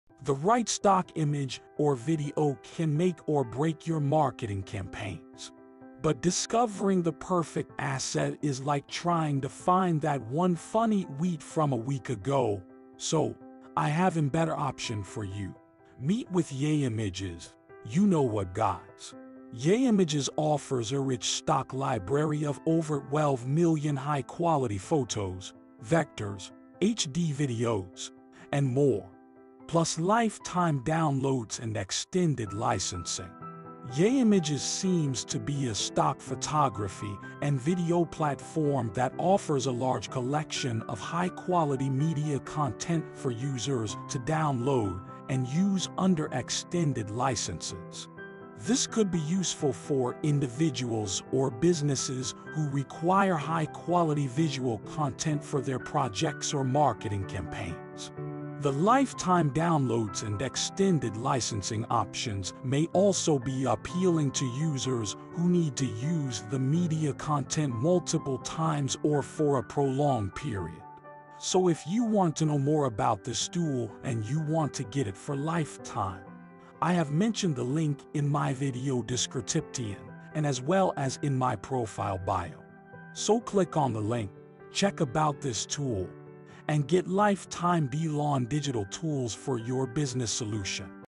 Yay Images Review | Yayimages Stock photography agency - Review and how to upload
So, in this video, I've presented the Yay Images
 Review:

What Is Yay Images:
The right stock image or video can make or break your marketing campaigns.

But discovering the perfect asset is like trying to find that one funny tweet from a week ago. (“Which thread had that Padme and Anakin meme about sandwich meat?”)

Say goodbye to endless searching, confusing licensing, and expensive content.

Best Hosting Providers Are Available here:-

✅Some Important Playlists Are Also Available For You here:-

✅Highly Searching Keywords:-